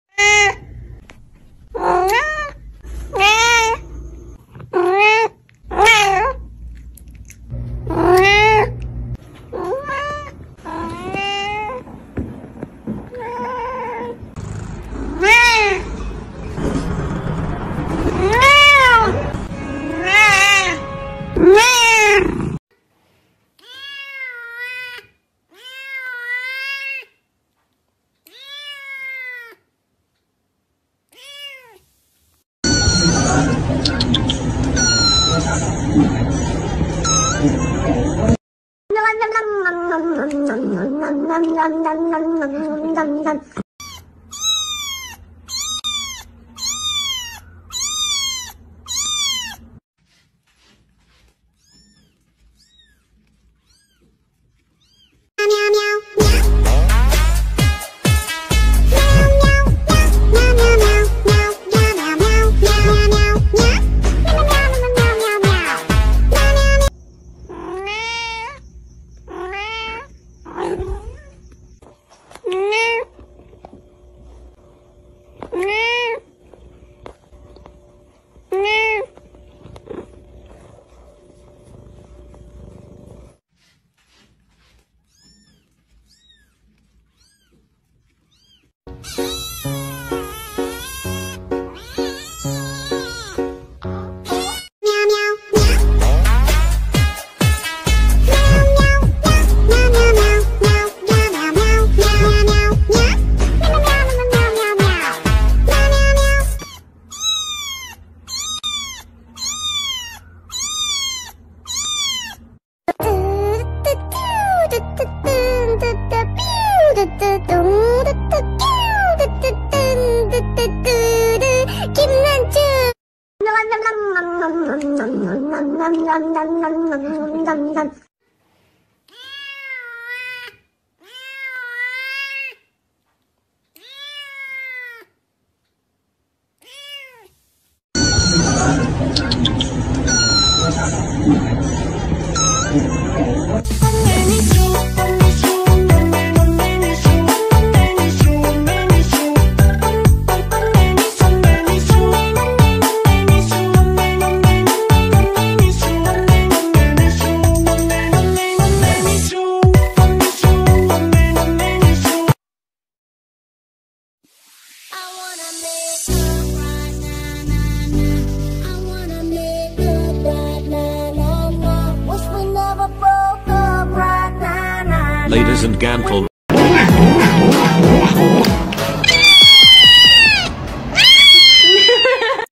suara anak kucing mengeong,di jamin induknya langsung datang
Suara anak kucing mengeong kecil dan bicara-kitten meowing sound